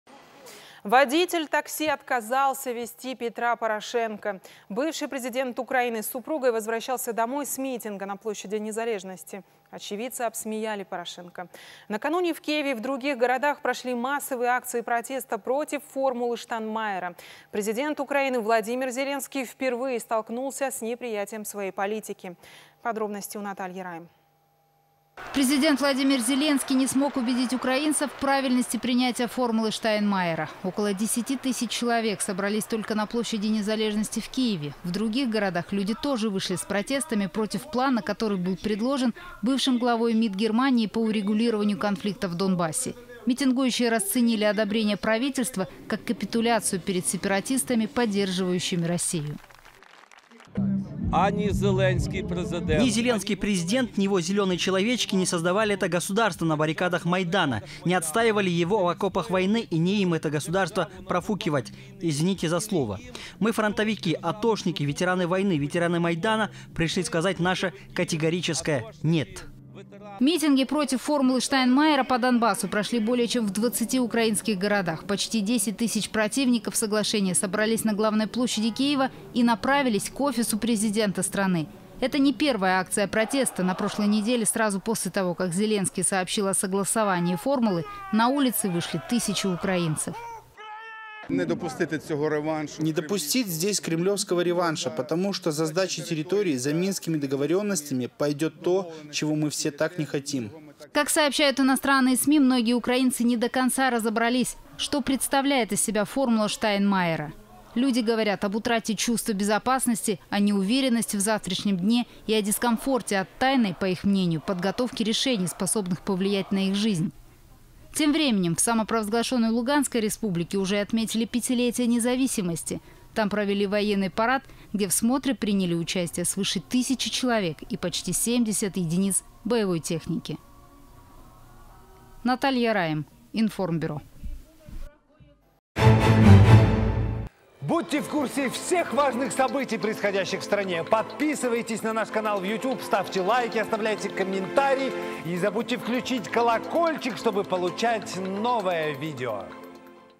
На Украине проходят массовые протесты против «формулы Штайнмайера»
Водитель такси отказался везти Петра Порошенко. Бывший президент Украины с супругой возвращался домой с митинга на площади Незалежности. Очевидцы обсмеяли Порошенко. Накануне в Киеве и других городах прошли массовые акции протеста против «формулы Штайнмайера».  Президент Украины Владимир Зеленский впервые столкнулся с неприятием своей политики.